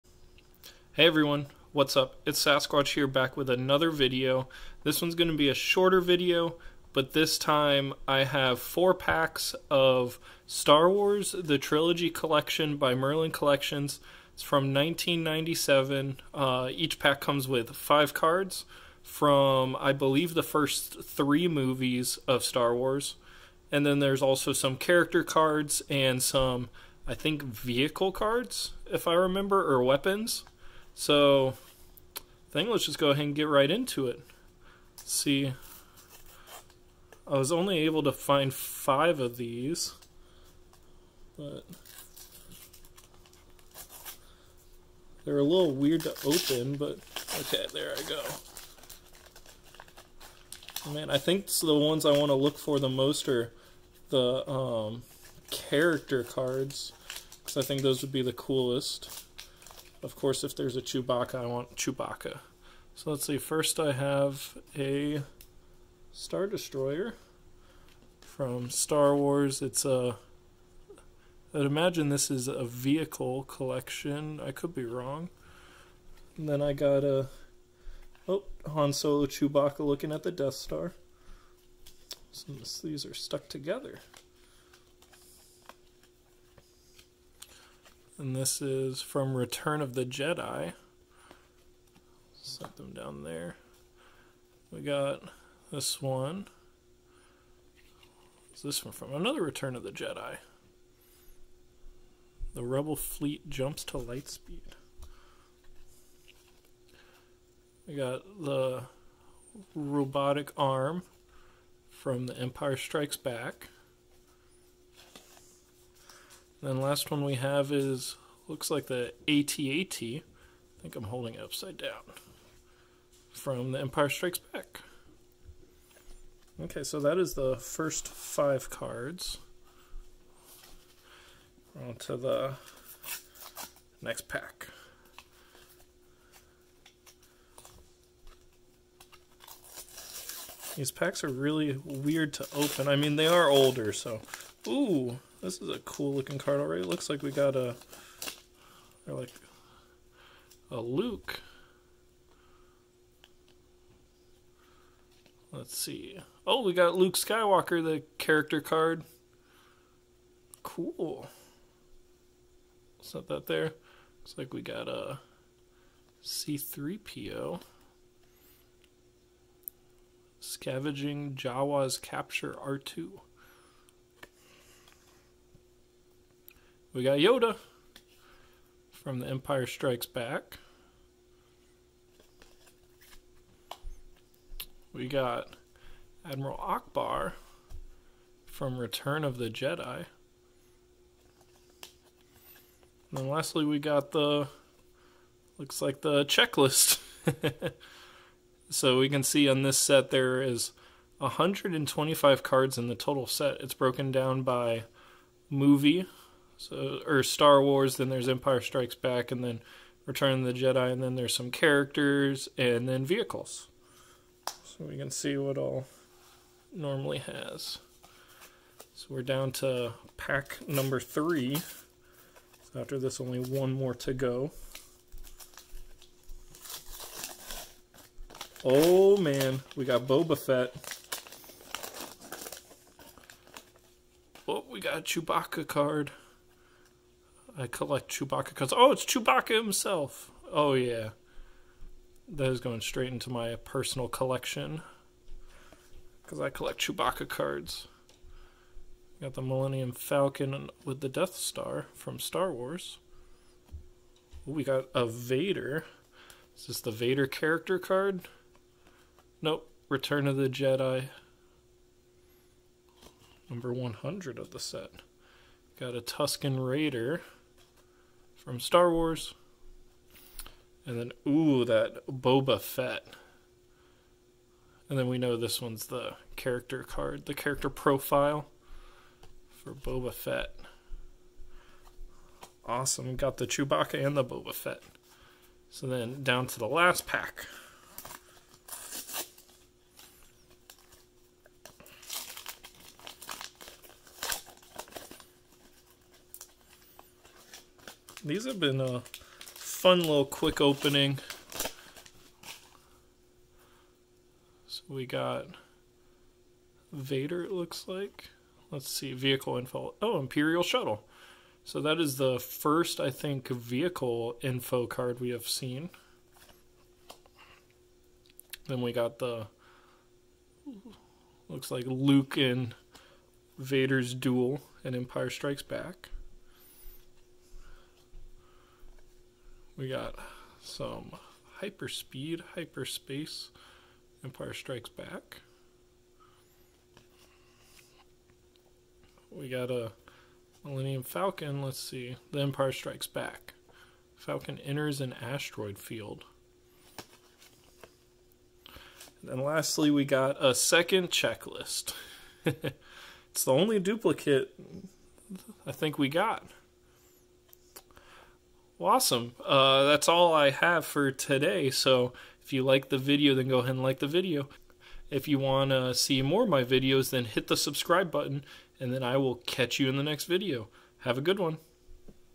Star Wars Trilogy Collection | Mini Pack Opening
Hey everyone! I saw these loose packs in a local store and had to pick them up. It is always an adventure opening up old packs like this!